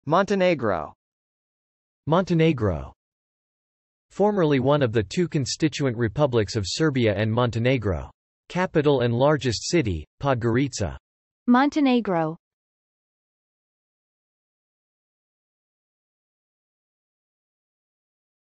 MONTENEGRO - Meaning and Pronunciation
How to pronounce montenegro? This video provides examples of an American pronunciation of montenegro from two male and two female speakers.
In addition, it explains the meaning of montenegro through a ditionary definition and several visual examples.

IPA Transcription of montenegro: /mˌɑntənˈeɪgrə/
Definition of montenegro: , formerly one of the two constituent republics of Serbia and Montenegro. Capital and largest city: Podgorica.

If you'd like to test your pronunciation of montenegro, check out Accent Hero's real-time pronunciation feedback tool: